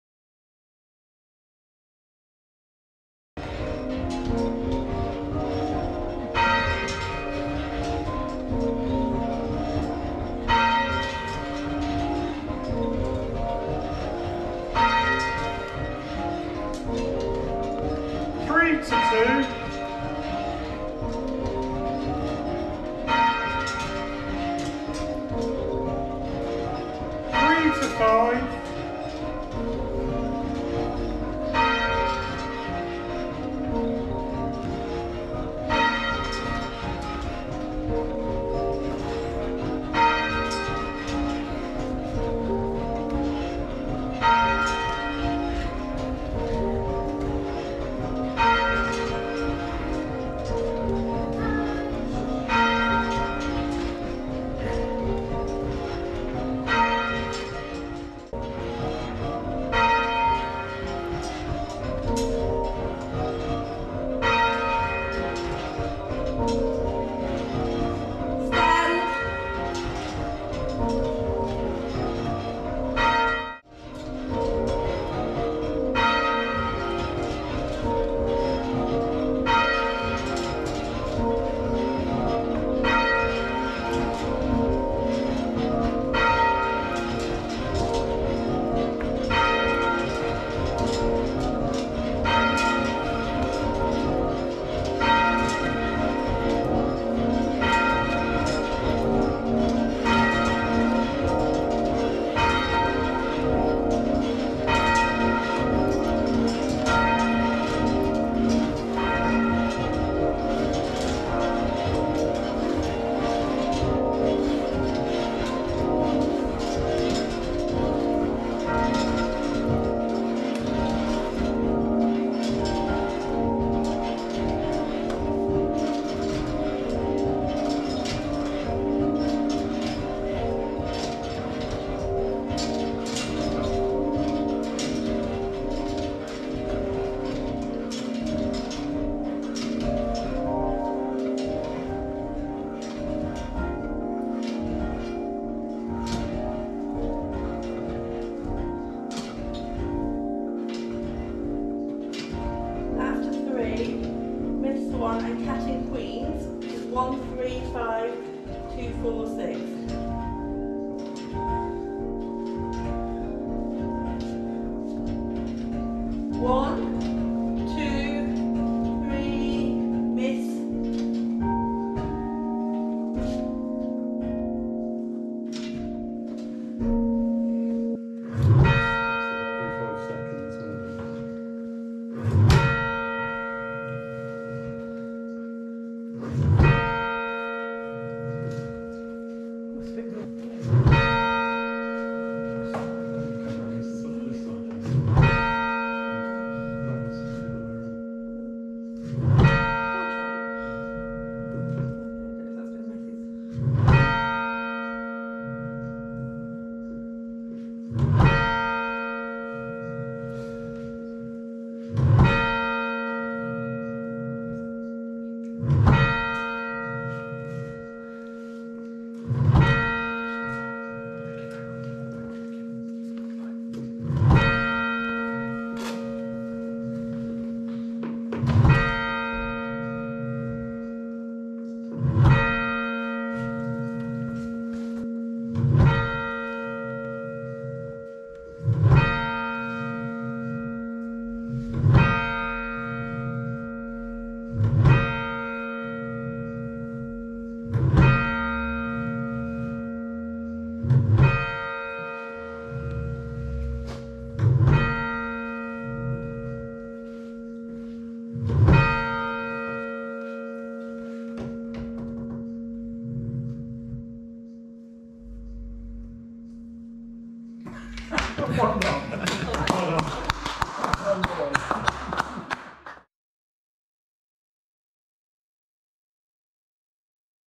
Ringing for the Funeral of The Queen
Ringing at St. Peters, Wolvercote fully muffled with the Tenor 'open' at Backstroke to mark the Funeral of The Queen.